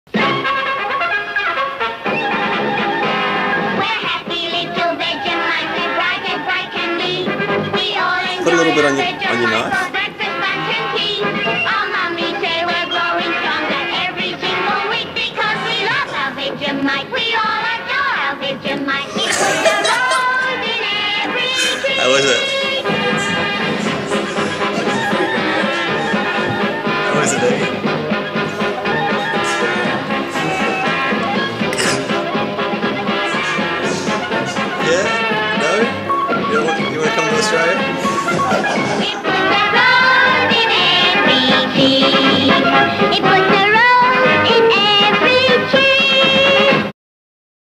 Vegemite Taste Test: Not A Happy Little Vegemite
I had my missus try Vegemite for the first. Her reaction... Priceless! :D
She's also having a crack at a modelling career, Check out her FB profile and show her some support